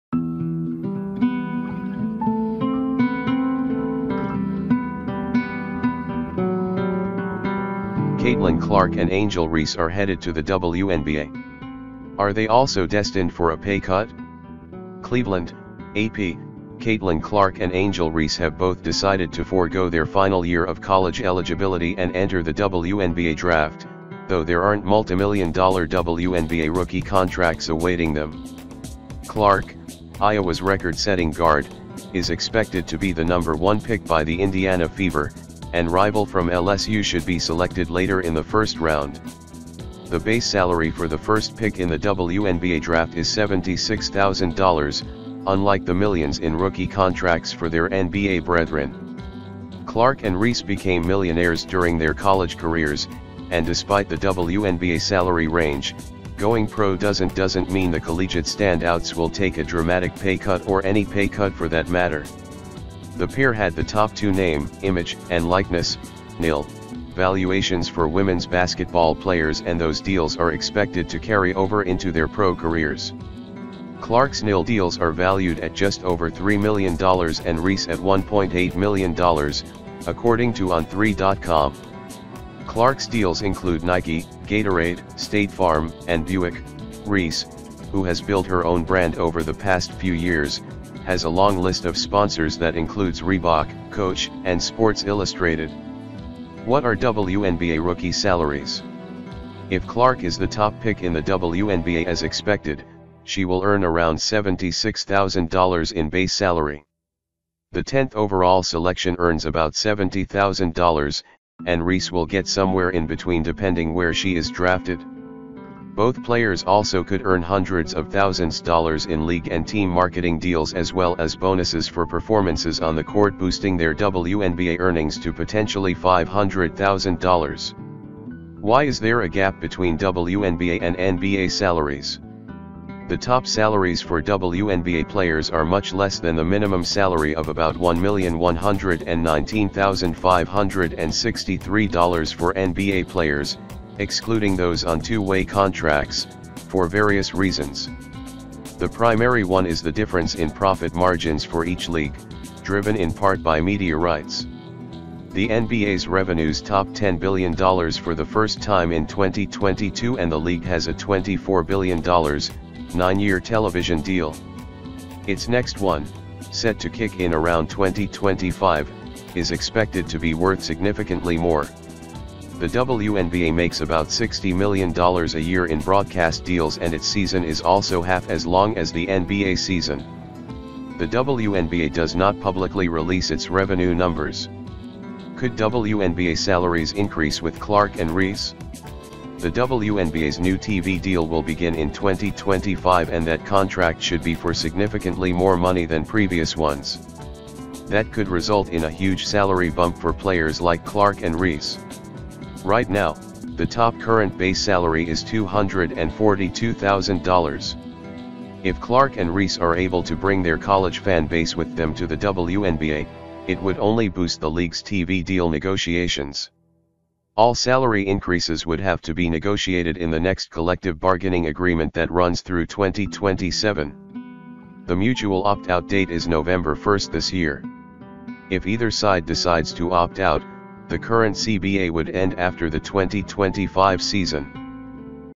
🔴Caitlin Clark and Angel Reese are headed to the WNBA. Are they also destined for a pay cut?🔴
CLEVELAND (AP) — Caitlin Clark and Angel Reese have both decided to forgo their final year of college eligibility and enter the WNBA Draft, though there aren’t multimillion dollar WNBA rookie contracts awaiting them.

Clark, Iowa’s record-setting guard, is expected to be the No. 1 pick by the Indiana Fever, and rival from LSU should be selected later in the first round. The base salary for the first pick in the WNBA draft is $76,000, unlike the millions in rookie contracts for their NBA brethren.

Clark and Reese became millionaires during their college careers, and despite the WNBA salary range, going pro doesn’t doesn’t mean the collegiate standouts will take a dramatic pay cut — or any pay cut for that matter.

The pair had the top two name, image and likeness (NIL) valuations for women’s basketball players and those deals are expected to carry over into their pro careers.

Clark’s NIL deals are valued at just over $3 million and Reese at $1.8 million, according to On3.com. Clark’s deals include Nike, Gatorade, State Farm and Buick; Reese, who has built her own brand over the past few years, has a long list of sponsors that includes Reebok, Coach and Sports Illustrated.

WHAT ARE WNBA ROOKIE SALARIES?
If Clark is the top pick in the WNBA as expected, she will earn around $76,000 in base salary. The 10th overall selection earns about $70,000, and Reese will get somewhere in between — depending where she is drafted. Both players also could earn hundreds of thousands dollars in league and team marketing deals as well as bonuses for performances on the court — boosting their WNBA earnings to potentially $500,000.

WHY IS THERE A GAP BETWEEN WNBA AND NBA SALARIES?
The top salaries for WNBA players are much less than the minimum salary of about $1,119,563 for NBA players (excluding those on two-way contracts) for various reasons. The primary one is the difference in profit margins for each league, driven in part by media rights. The NBA’s revenues topped $10 billion for the first time in 2022 and the league has a $24 billion, nine-year television deal. Its next one, set to kick in around 2025, is expected to be worth significantly more. The WNBA makes about $60 million a year in broadcast deals and its season is also half as long as the NBA season. The WNBA does not publicly release its revenue numbers.

COULD WNBA SALARIES INCREASE WITH CLARK AND REESE?
The WNBA’s new TV deal will begin in 2025 and that contract should be for significantly more money than previous ones. That could result in a huge salary bump for players like Clark and Reese. Right now, the top current base salary is $242,000.

If Clark and Reese are able to bring their college fan base with them to the WNBA, it would only boost the league’s TV deal negotiations.

All salary increases would have to be negotiated in the next collective bargaining agreement that runs through 2027. The mutual opt out date is Nov. 1 this year. If either side decides to opt out, the current CBA would end after the 2025 season.